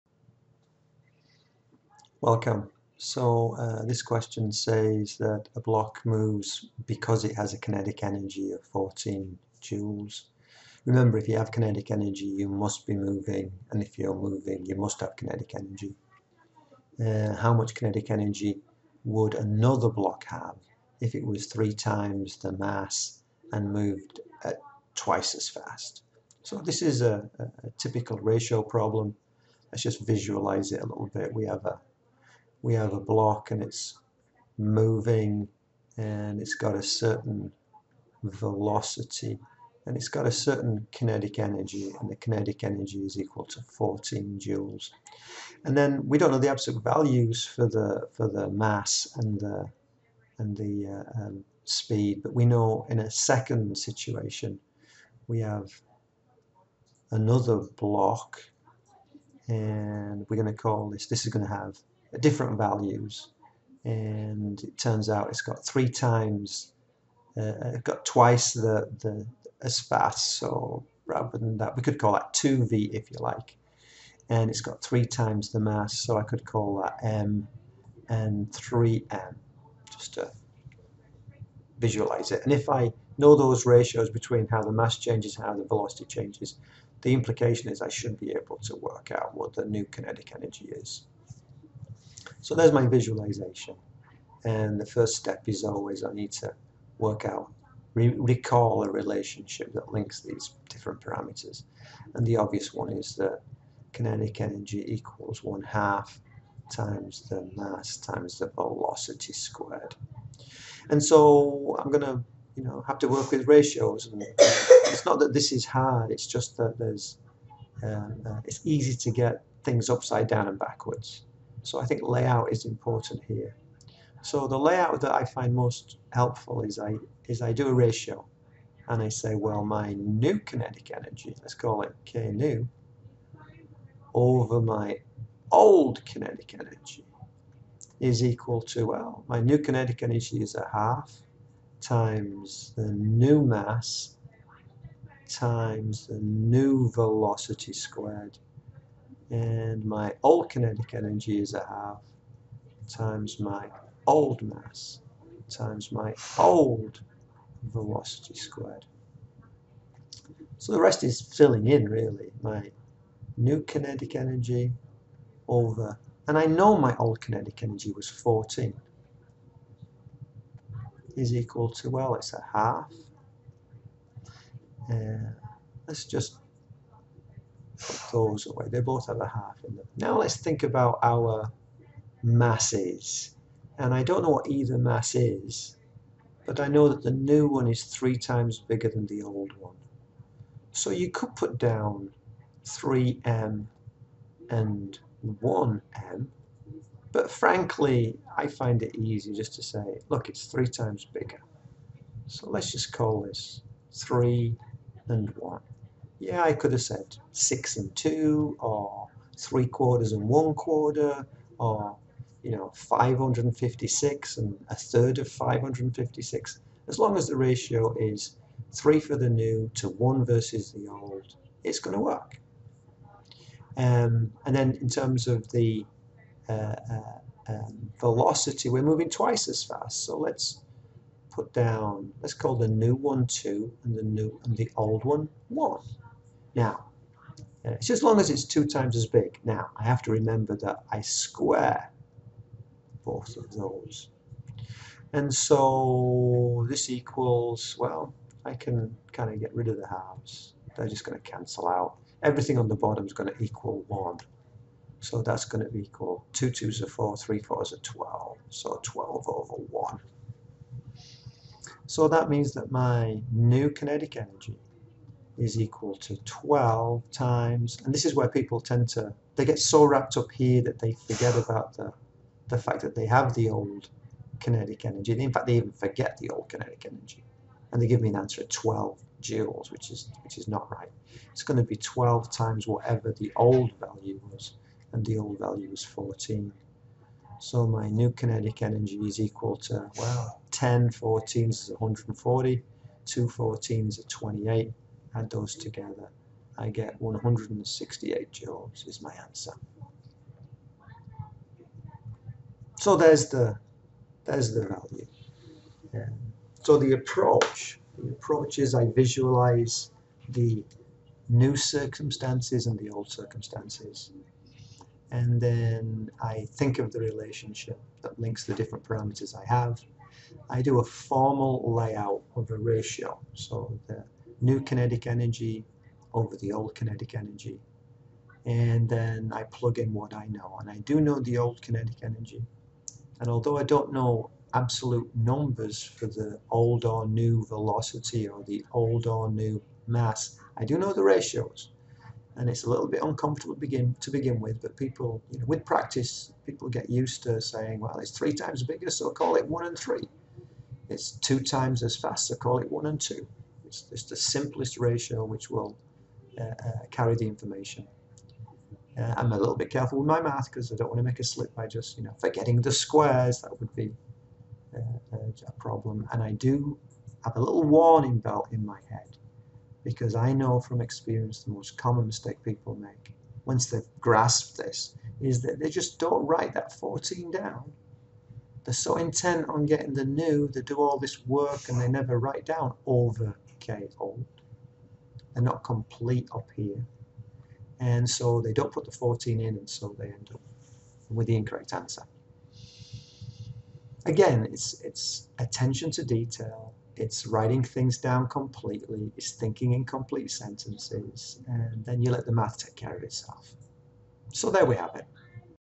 8) Kinetic Energy 1 - ratio
Frod new kinetic energy given the ratio of change for the mass and speed of a block.